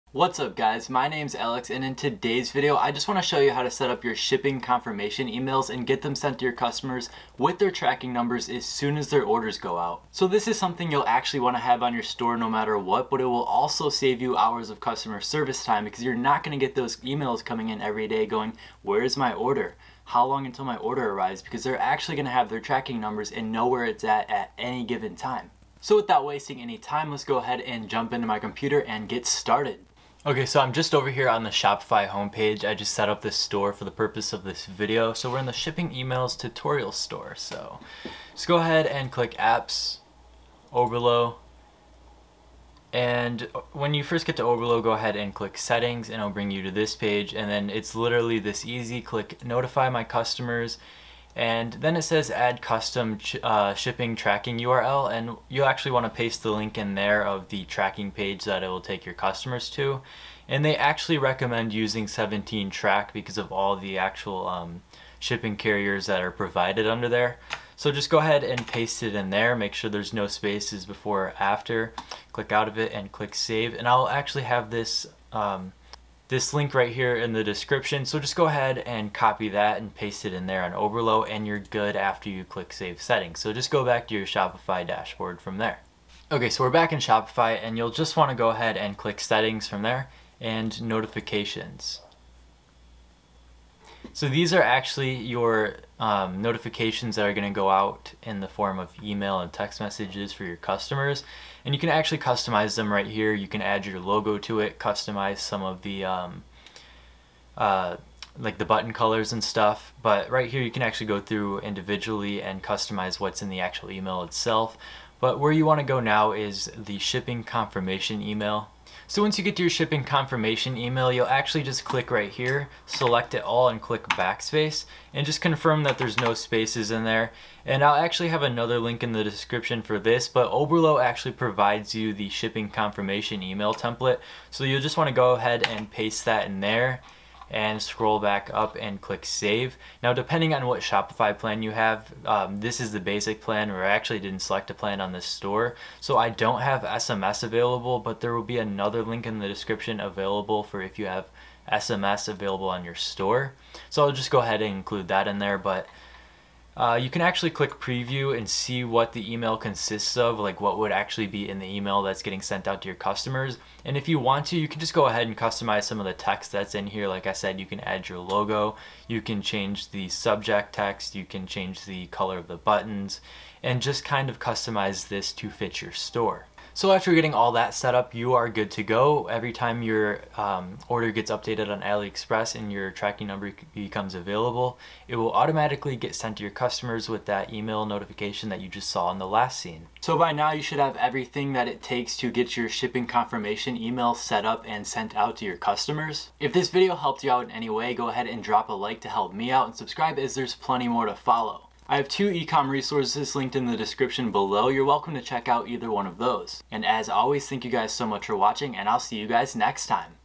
How To Set Up Shipping Confirmation Emails With Tracking In Oberlo | Shopify Dropshipping 2022
In this video, I explain how to set up your customer's shipping confirmation emails and get their tracking number delivered as soon as each order is shipped out.

Let Connect!